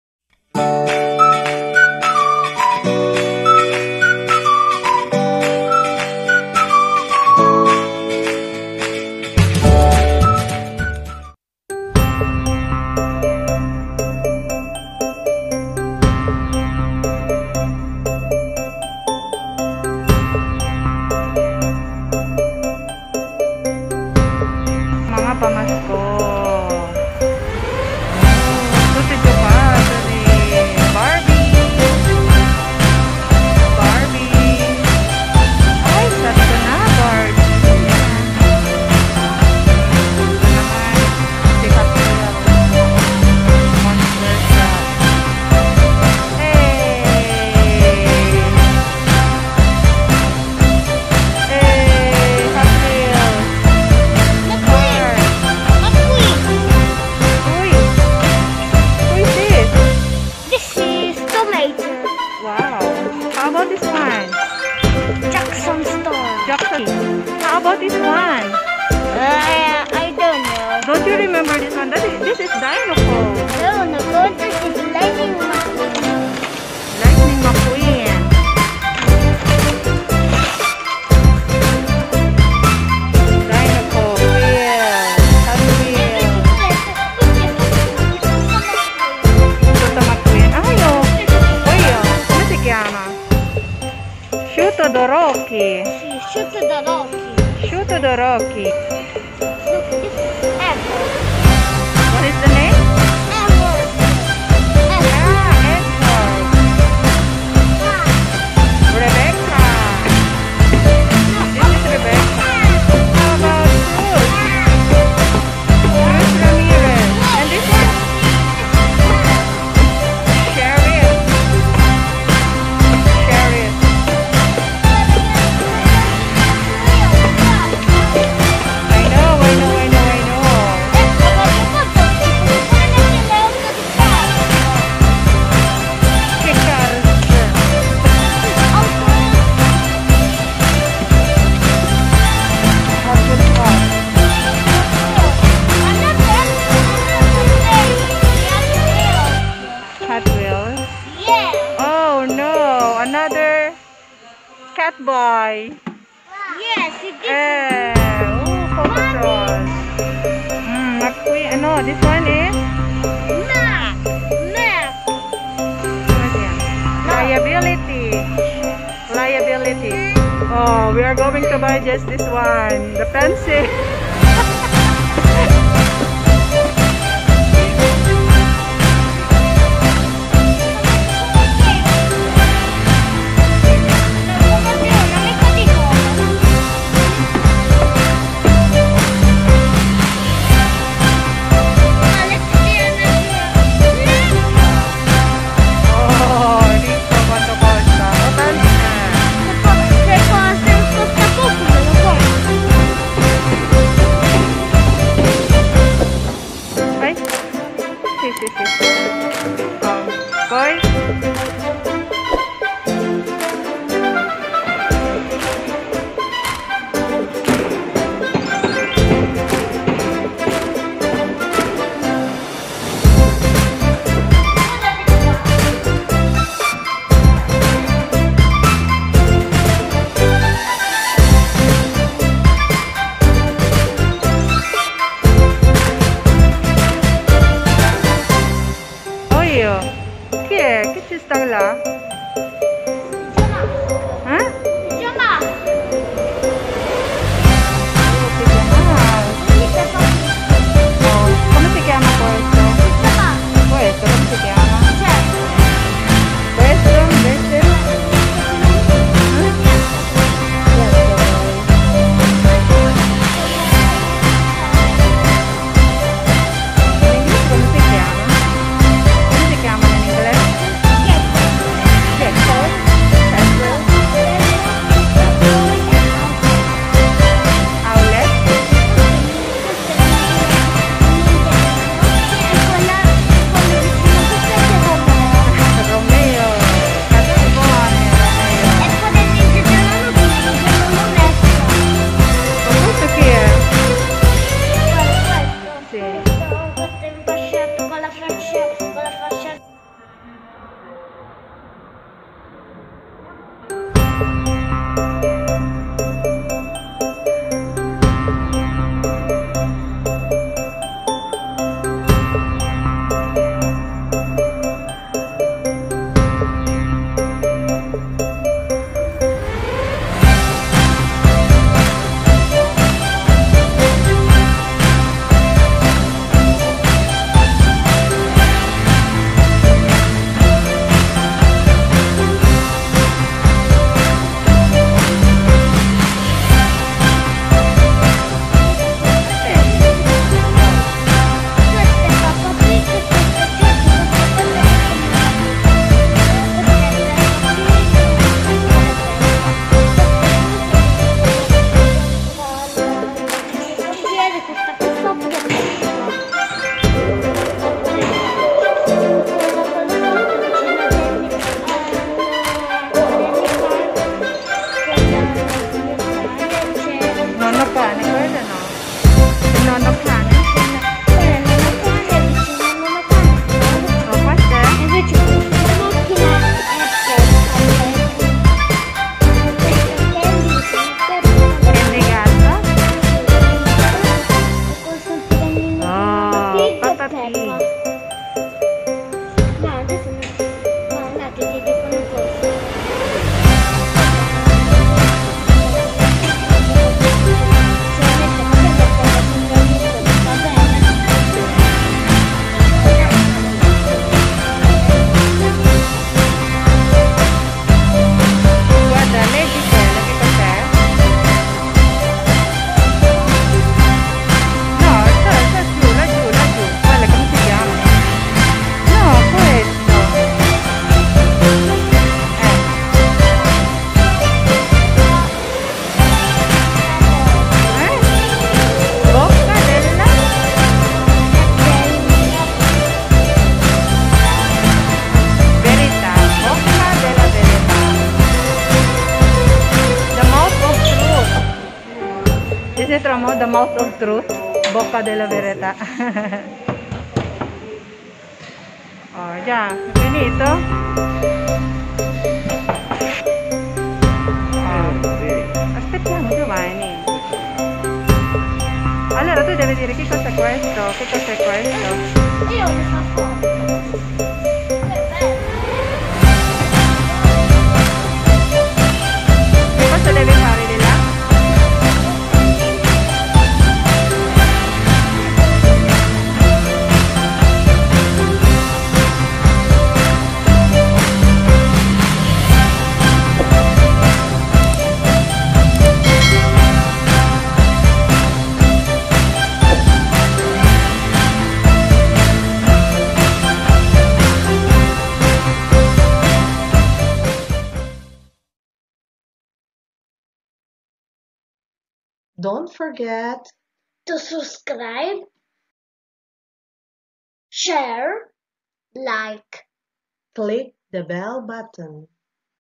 McQueen and friends - Saetta McQueen e gli amici/Giovanni's Play and Learn TV
Buying some stuff with my family at the mall so I had the chance to roam around and go to my favorite section which is the toys section of the mall.

Sit back, relax and enjoy watching.